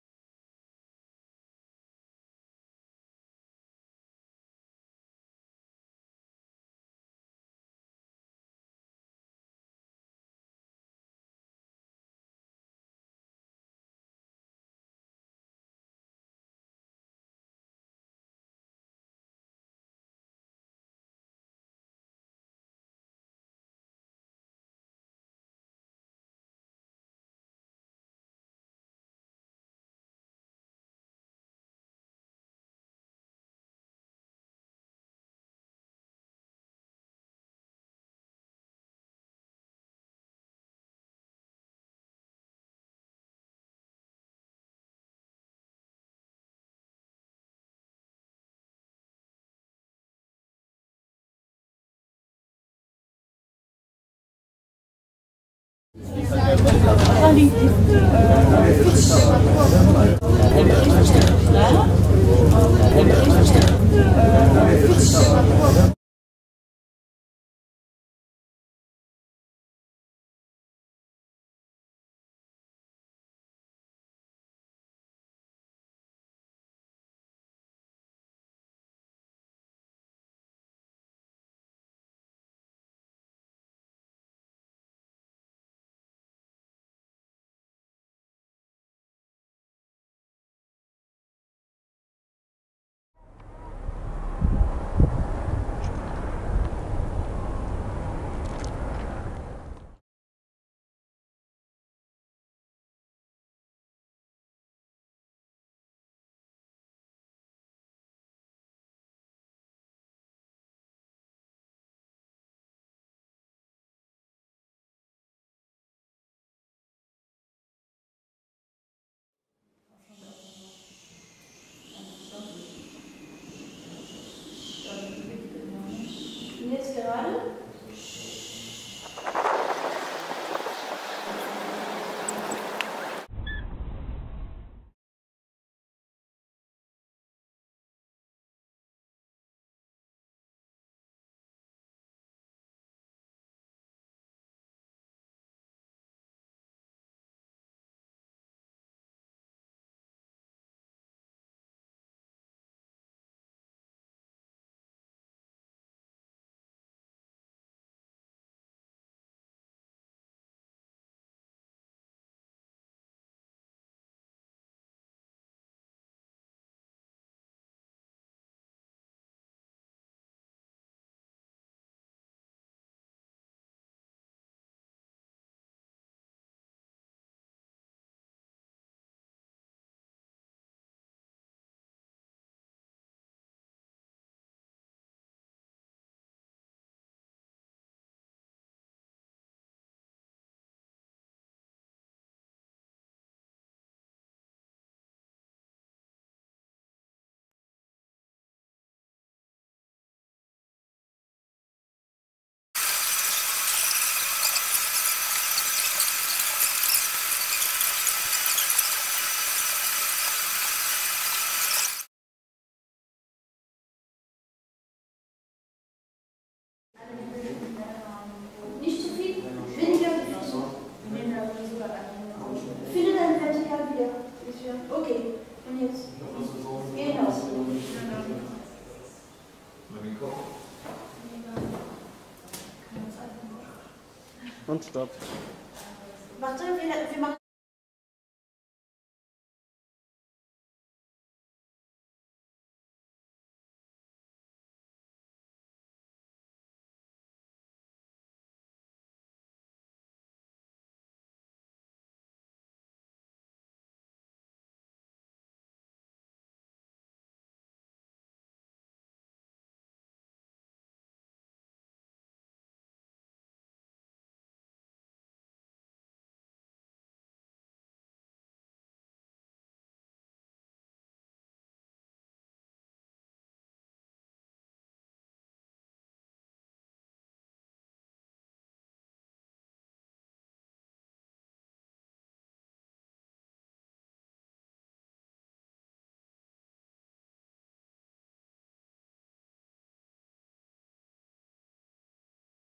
24 Hours - Berlin In Bewegung
Dieser Film ist als ein Porträt Berlins gedacht und gewährt so Einblicke in Berlin rund um die Uhr - 24 Hours. Entstanden ist dieser überwiegend bildbasierte Kurzfilm während eines Medienseminars, dass in Berlin von dem Fokus e.V. Osnabrück durchgeführt wurde. Als Producer kann man LKOS Media, das sind Florian Köster, Pablo Guido, Nico Steiger und Jan Hermeling, bezeichnen.

Wir hoffen, dass dieser Kurzfilm auf großes Interesse stößt und vielleicht dem Einen oder Anderen Sehnsucht nach Berlin verleiht.

Viel Spaß beim Ansehen des Fims.